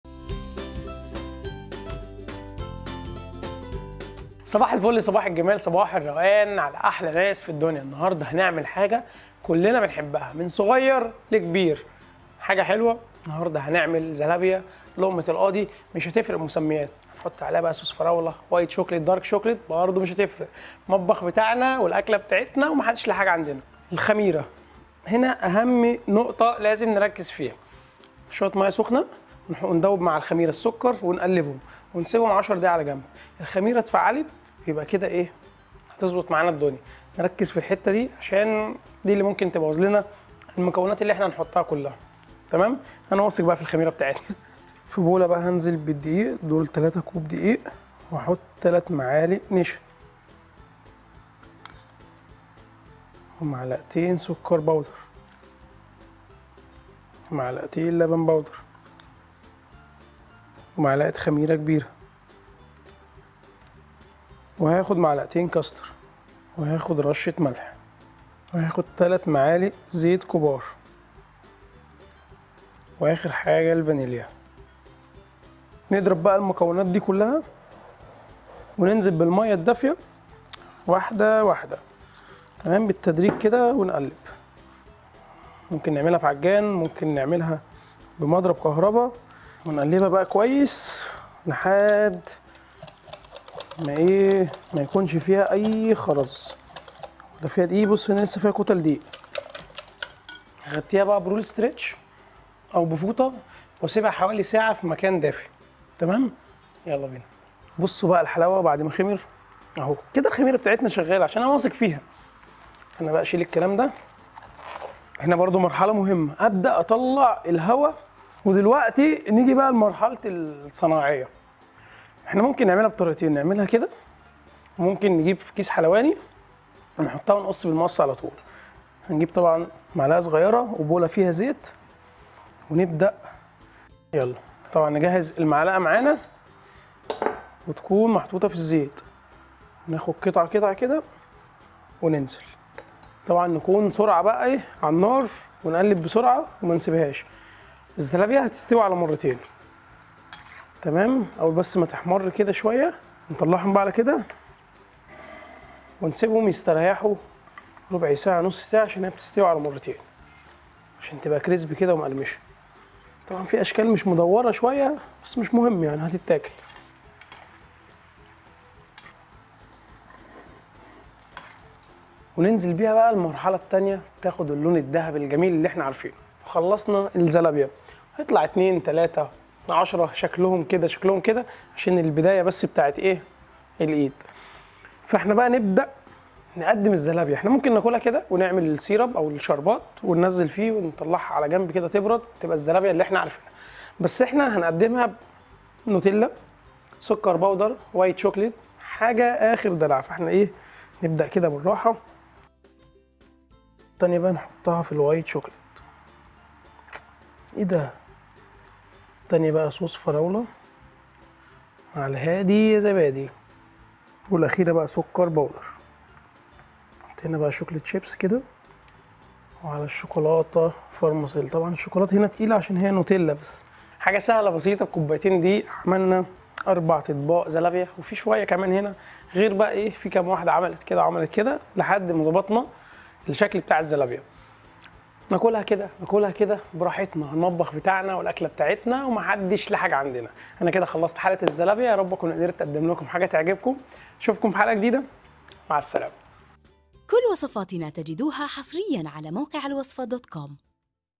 زلابية او لقمة القاضي على حسب انت تعرفها ازاي الشيف عاطف هيورينا سر طريقة عمل الزلابية
طريقة عمل الزلابية او لقمة القاغضي

 الوصفة عندنا!

المقادير
3 كوب دقيق
2 كوب ماء دافئ
3 معلقة كبيرة نشاء
1 معلقة كبيرة كاستر
1 معلقة كبيرة خميره فوريه
2 معلقة كبيرة سكر
1 معلقة صغيرة فانيليا
2 معلقة كبيرة حليب بودر
3 معلقة كبيرة زيت.
رشة ملح
للتقديم
صوص فراوله
سكر بودره
صوص شيكولاتة